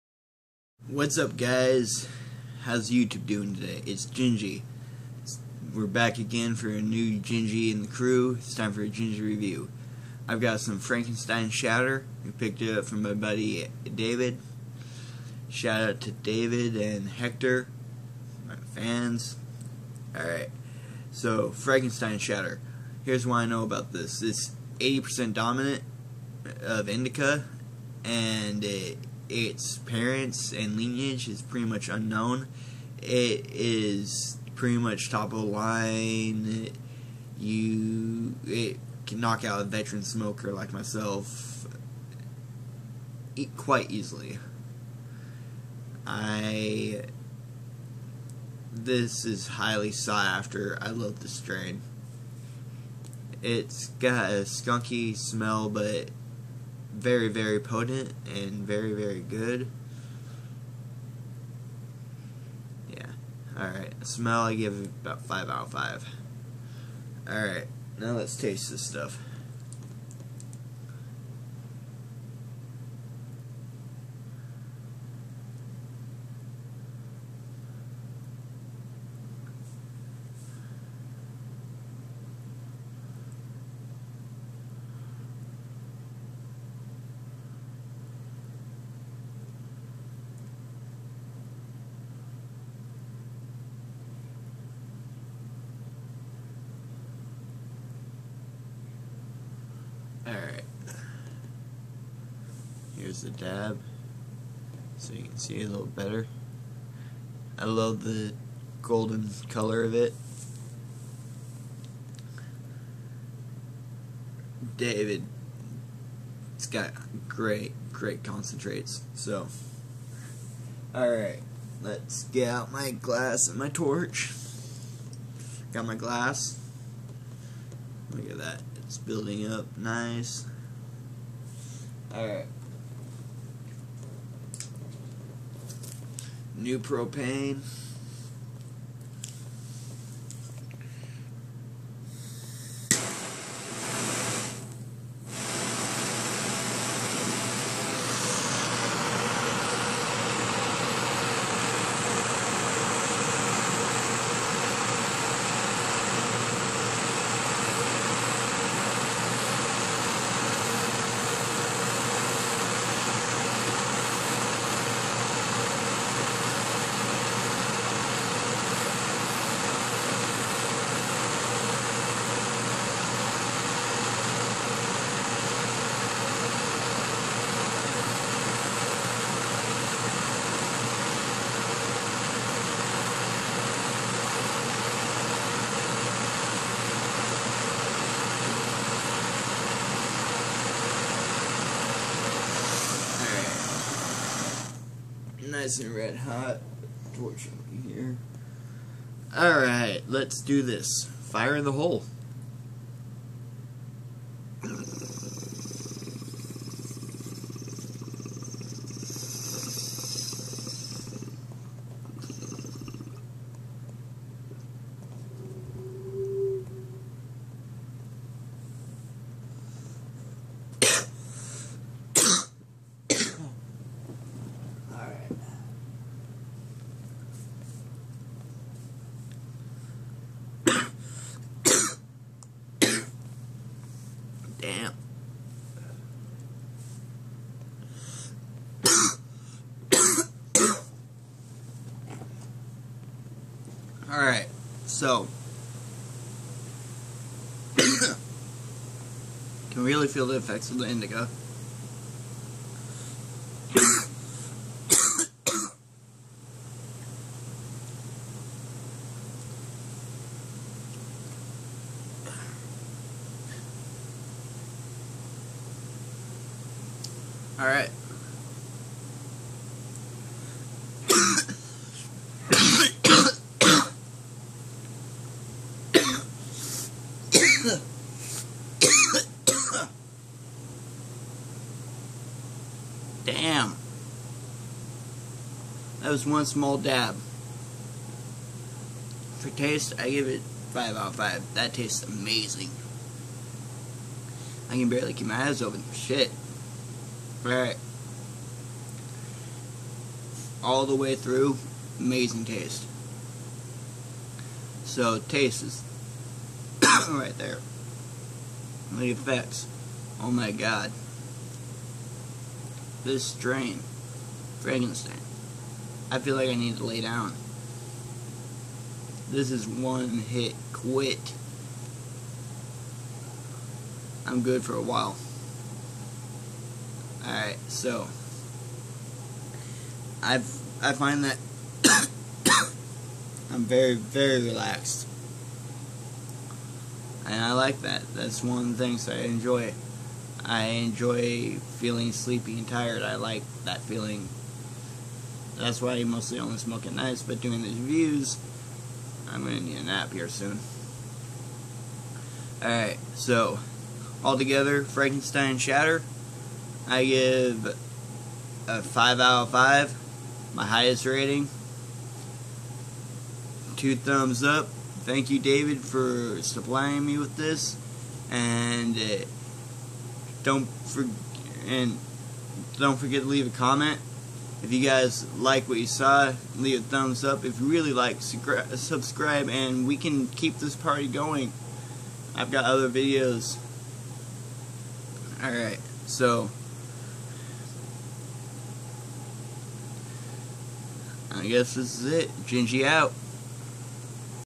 Frankenstein shatter Review
In this video I review the highly sought after Frankenstein shatter!!! I picked this up from a friend of mine. Its a 80% indica dominant strain whos parents are unknown. Knocks out veteran smokers after one hit.